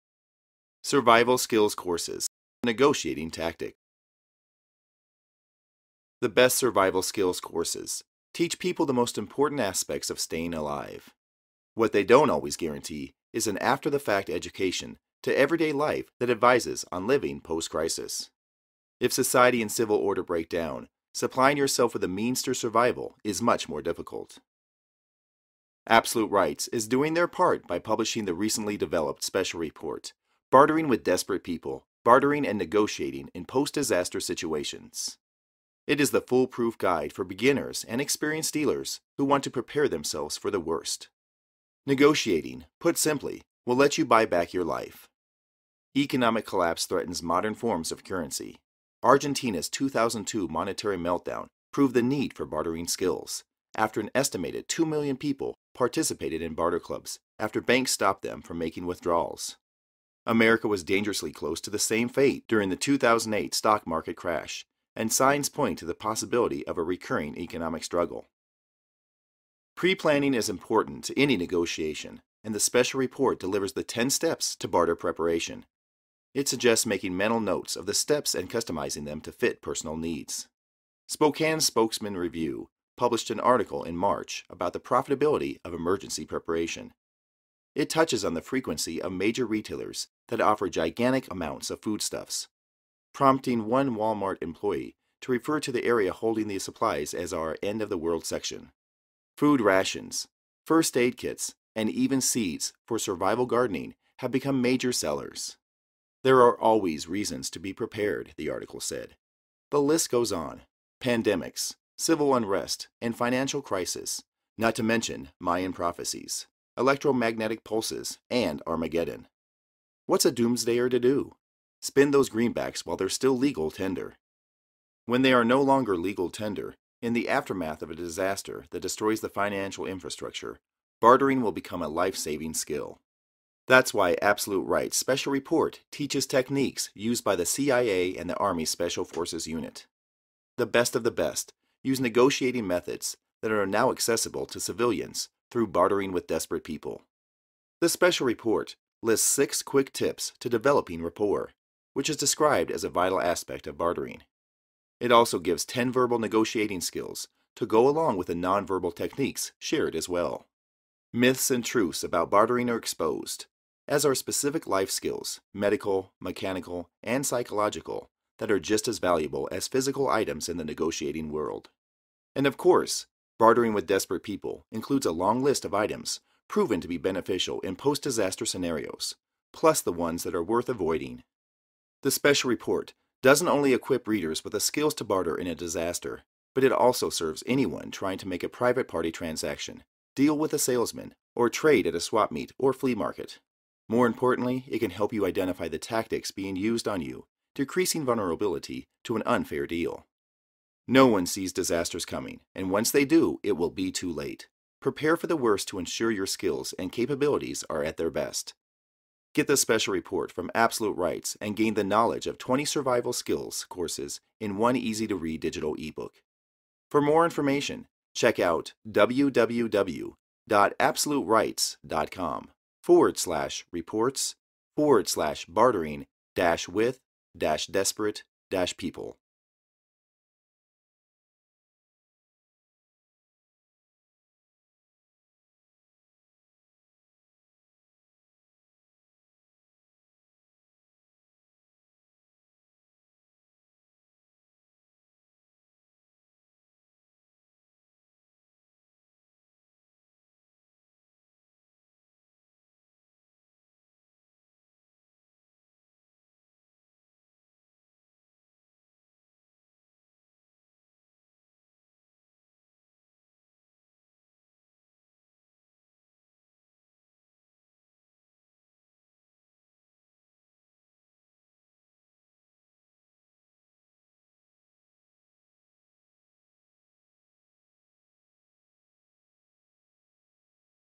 Survival Skills Courses: Staying Alive
In this video you'll learn how the best survival skills courses teach people the most important aspects of staying alive. What they don't always guarantee is an "after the fact" education to everyday life that advises on living post-crisis. If society and civil order breakdown, supplying yourself with the means to survival is much more difficult.